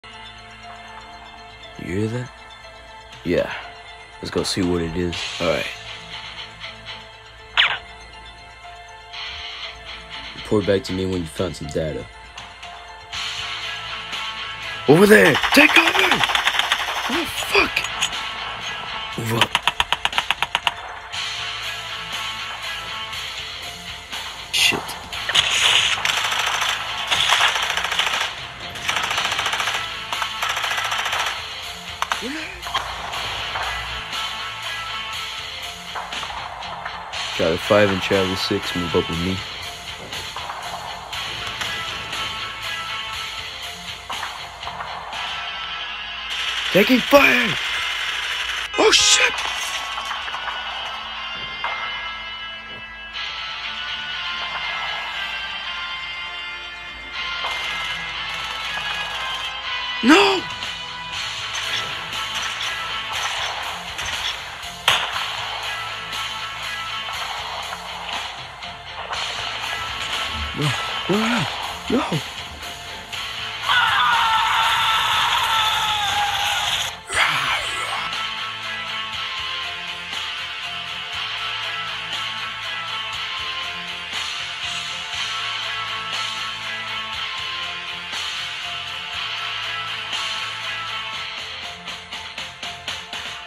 Undead Nation Pt.3
After their battle with the giant monster they venture out only to be ambushed by none other then the military. After their war they drove out of the battle while the rest of the marines are either wounded or dead